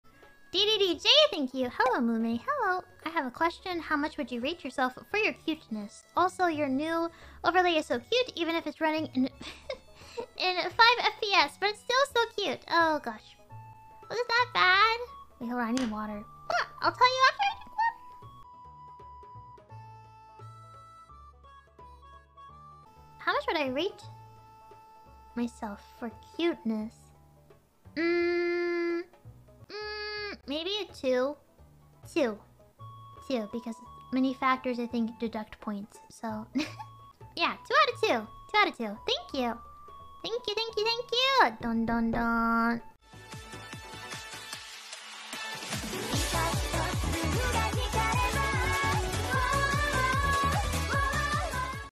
𝐌𝐮𝐦𝐞𝐢 𝐰𝐚𝐬 𝐚𝐬𝐤𝐞𝐝 𝐭𝐨 𝐫𝐚𝐭𝐞 𝐡𝐞𝐫 𝐜𝐮𝐭𝐞𝐧𝐞𝐬𝐬 |Hololive EN
Source:
ED ART by @Tkminnm from Twitter -

BGM used
DOVA-SYNDROME